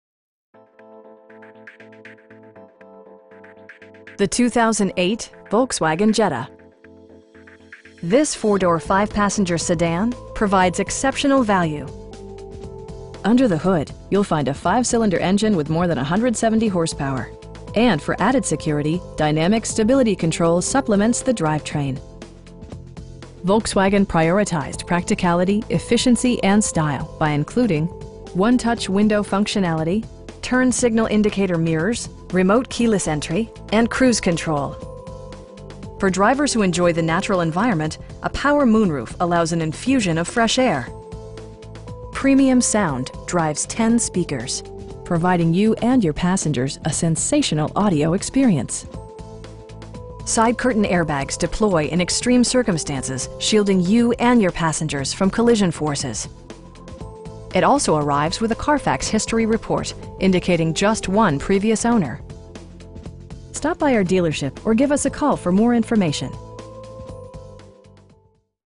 2008 Volkswagen Jetta in Fayetteville, NC 28303
Crown Dodge of Fayetteville

The 2008 Volkswagen Jetta. This 4 door, 5 passenger sedan provides exceptional value! Under the hood you'll find a 5 cylinder engine with more than 170 horsepower, and for added security, dynamic Stability Control supplements the drivetrain. Volkswagen prioritized practicality, efficiency, and style by including: 1-touch window functionality, tilt steering wheel, turn signal indicator mirrors, power windows, remote keyless entry, an overhead console, and cruise control. For drivers who enjoy the natural environment, a power moon roof allows an infusion of fresh air. Premium sound drives 10 speakers, providing you and your passengers a sensational audio experience. Side curtain airbags deploy in extreme circumstances, shielding you and your passengers from collision forces. It also arrives with a Carfax history report, indicating just one previous owner. We know that you have high expectations, and we enjoy the challenge of meeting and exceeding them! Stop by our dealership or give us a call for more information.